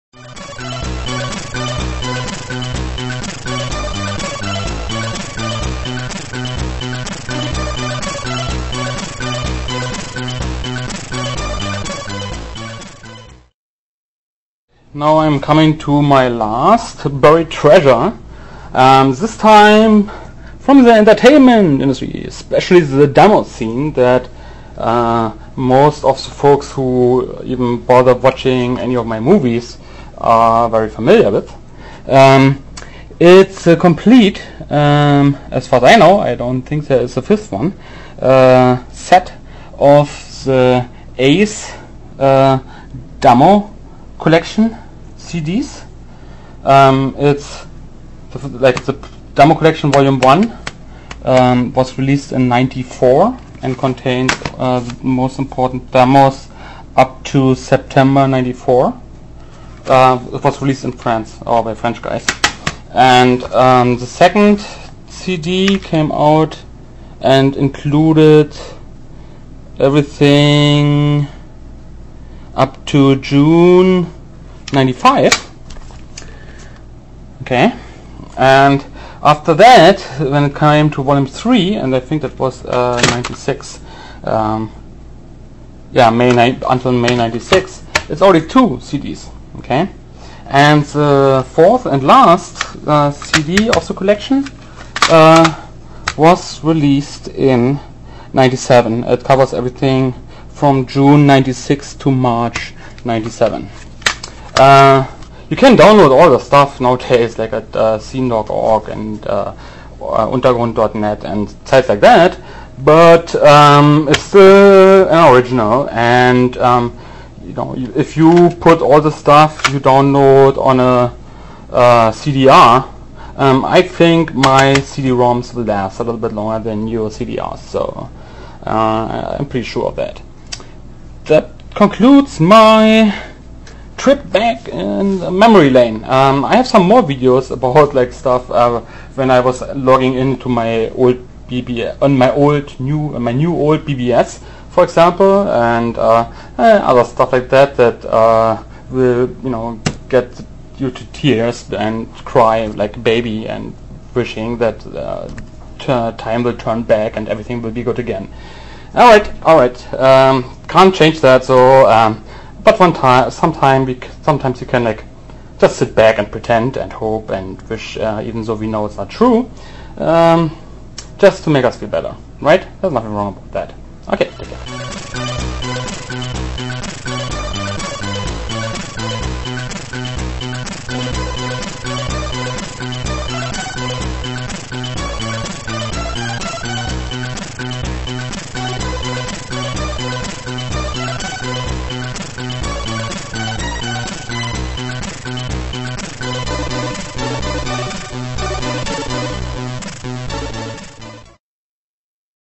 Trip Back on Memory Lane - Part 4 - ACE Demo Collection CD-ROMS
I created a mini-series about various scene related subjects. Each episode is made of a small video and a more detailed post at my personal blog.

This episode is about the French DEMO Collection CD-ROM series titled ACE

The Introduction and Summary post for the series can be found

Cheers!
Carsten aka Roy/SAC